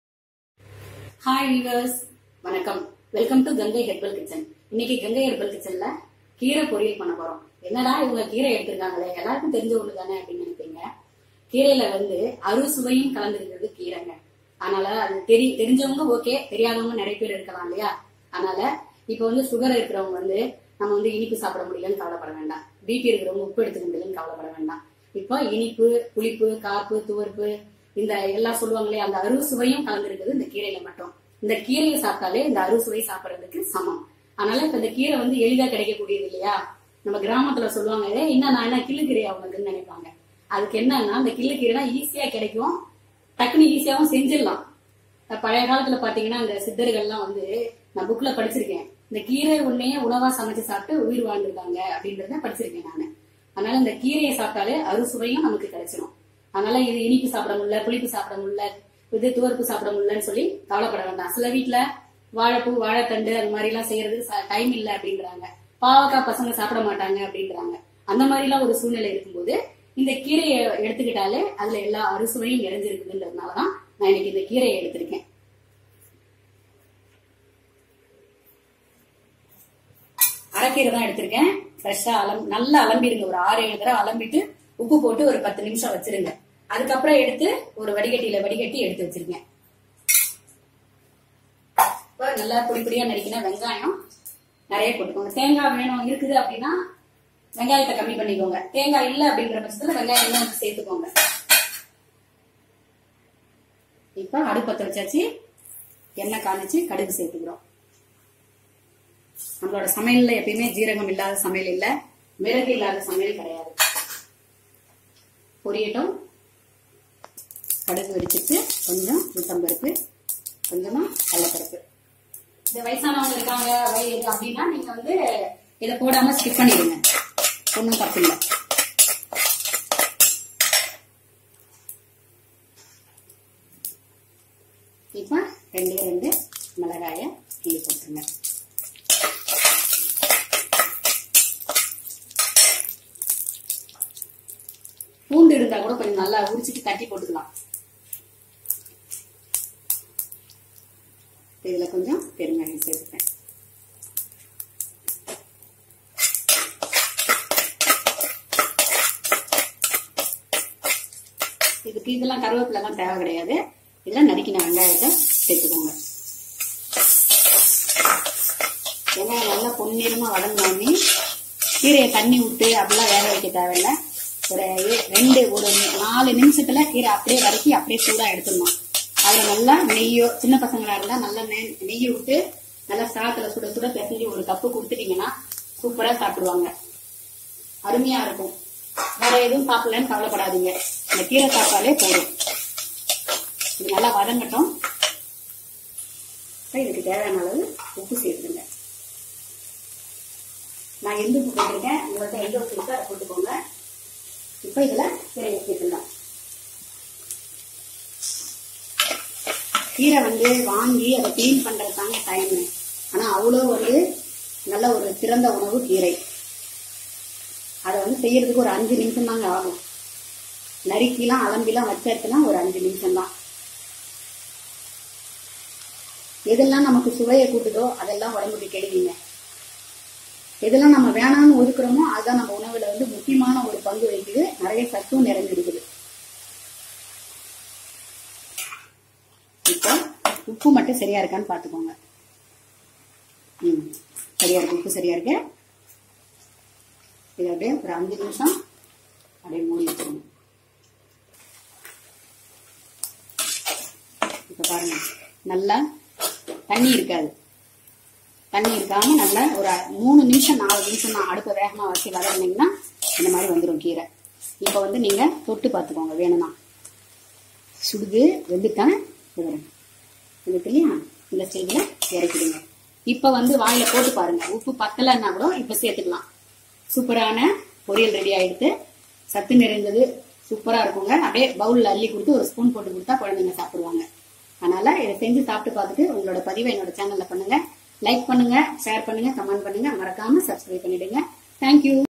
Arusavai migundha keerai/gangaiherbalkitchen/studentscancreate
This video is published on 8 May,2020
INGREDIENTS:
 araikeerai
 onion
 rocksalt
 oil
 mustard
jeeragam